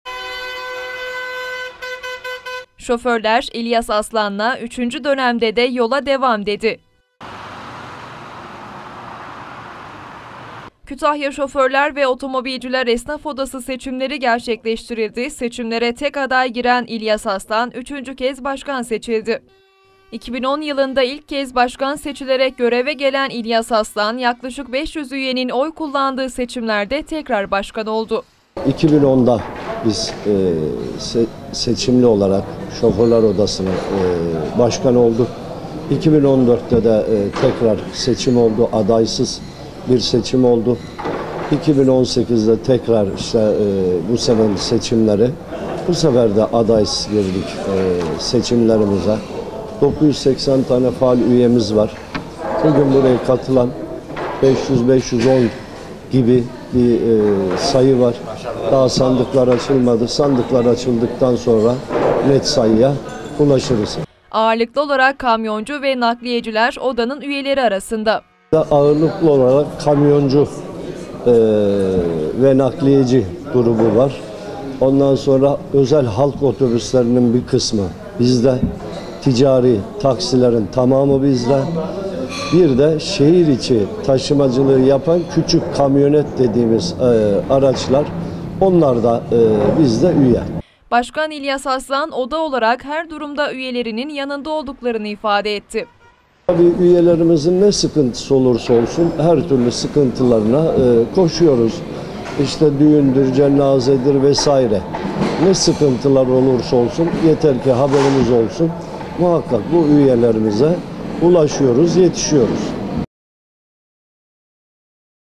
ŞÖFÖRLER ODASINDA 3. İLYAS ASLAN DÖNEMİ
Haber Metni:

KÜTAHYA ŞOFÖRLER VE OTOMOBİLCİLER ESNAF ODASI BAŞKANI İLYAS ASLAN, GÜVEN TAZELEDİ. GENEL KURULDA SEÇİMLERE TEK ADAY GİREN ASLAN, 3. KEZ BAŞKANLIK GÖREVİNİ YÜRÜTECEK.